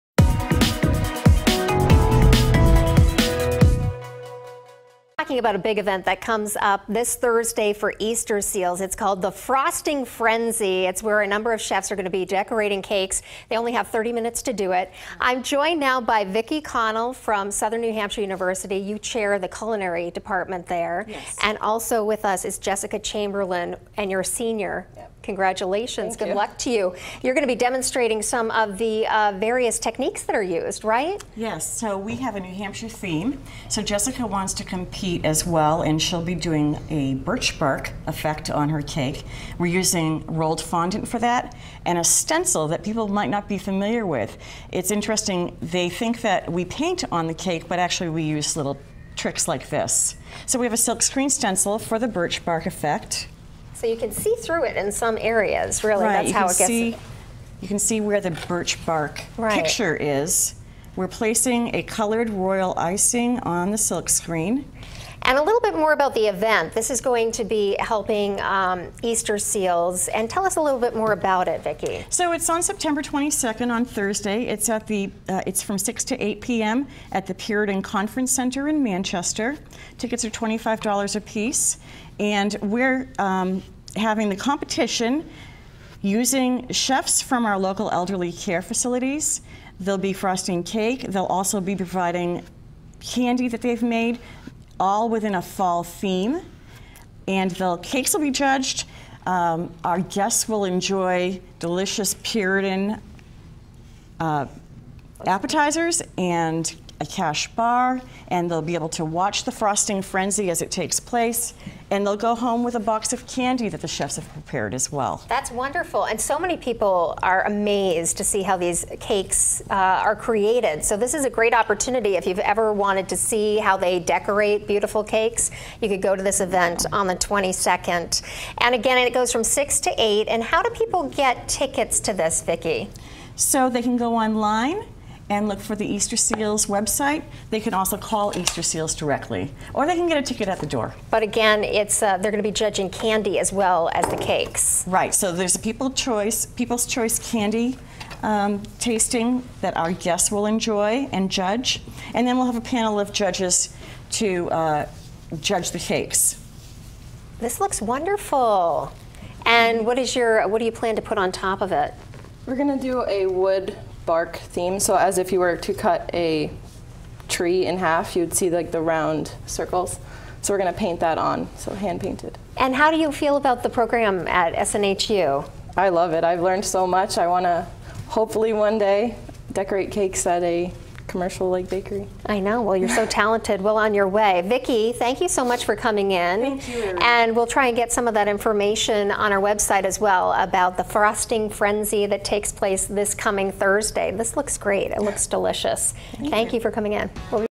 Frosting Frenzy - Cake decorating tips
Vicki Connell, SNHU culinary Department, and Jessica Chamberlin, SNHU culinary student, give Erin Fehlau some cake decorating tips.